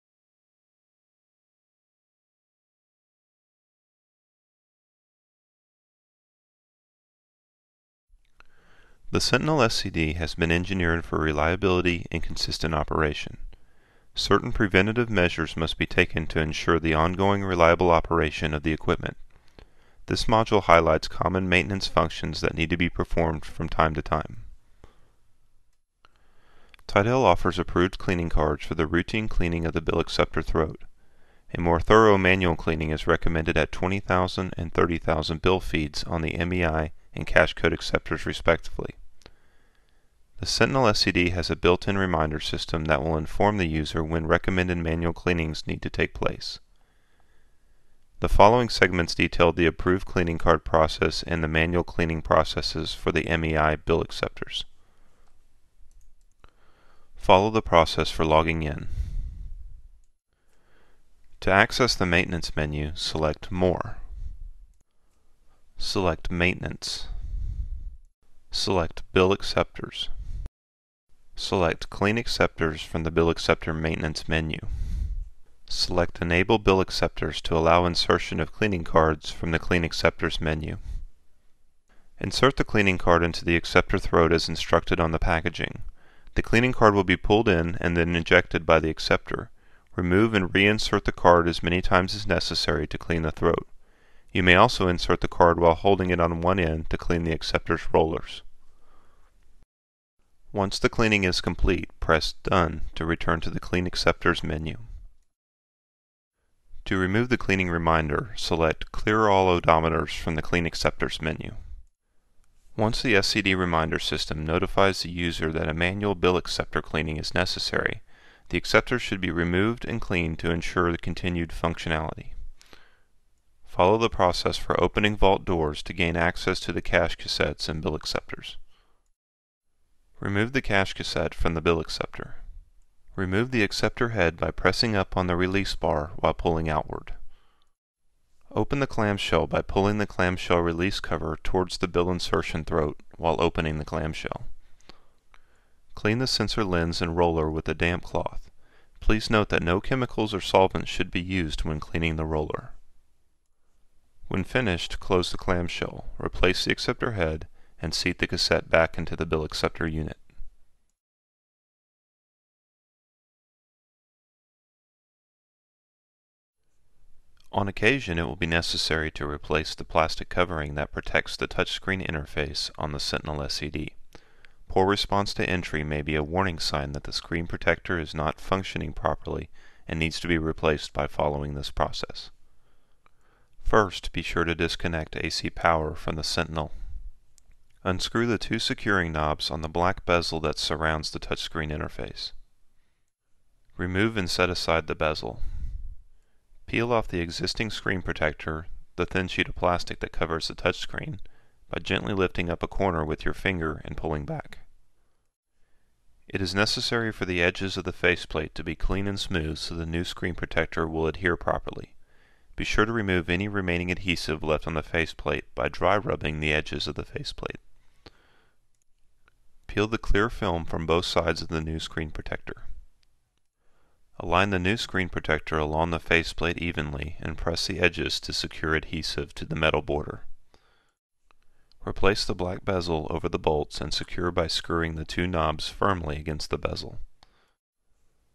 Tidel Sentinel SCD Training - Troubleshooting and Maintenance
1) Bill acceptor cleaning

2) Screen protector replacement